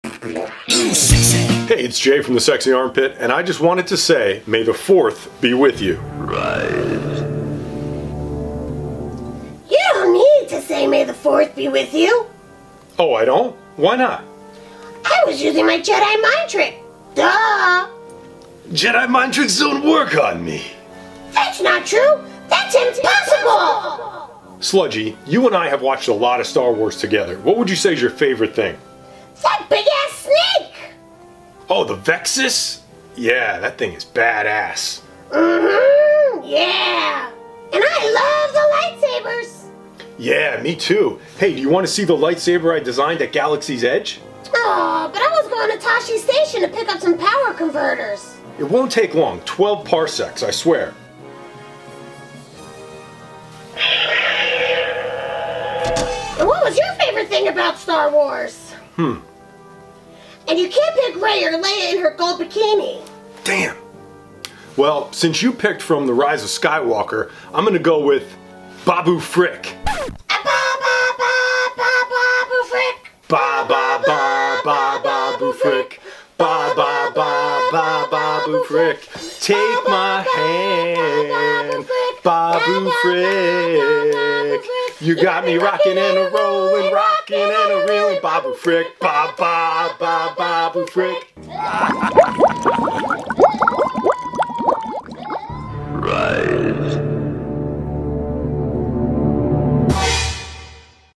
STAR WARS DAY GREETING 2020
In honor of Star Wars Day, Jay and Sludgey talk about their favorite things from Star Wars!